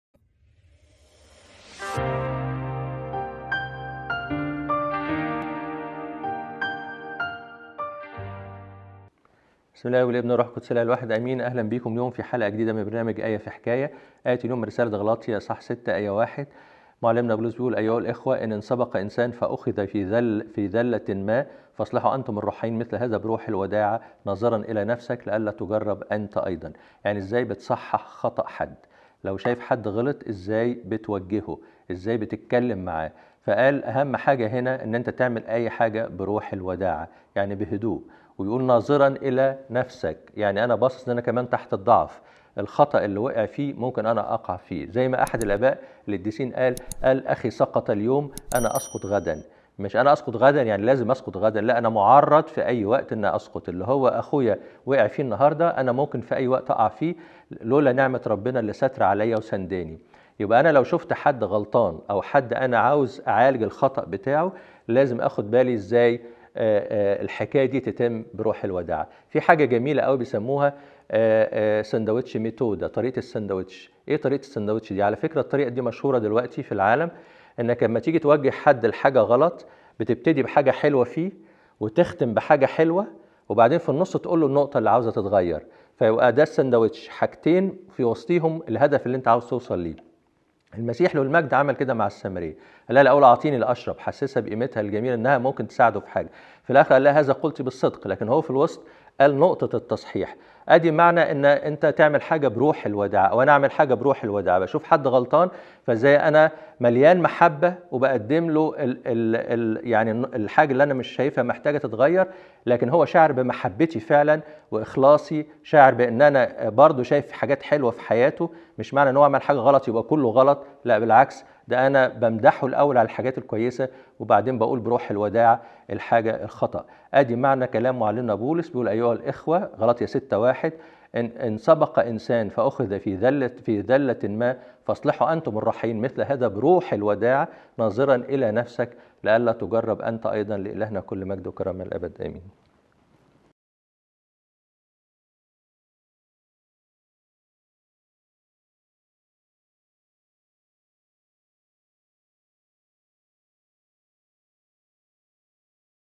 آية في حكاية - تصحيح الخطأ بروح الوداعة و تقديم المحبة
آية في حكاية - الحلقة 80 - تقديم القمص يسي ميساك
آية اليوم
أيها الإخوة، إن انسبق إنسان فأخذ في زلة ما، فأصلحوا أنتم الروحانيين مثل هذا بروح الوداعة، ناظرا إلى نفسك لئلا تجرب أنت أيضا
الرسالة إلى أهل غلاطية  1:6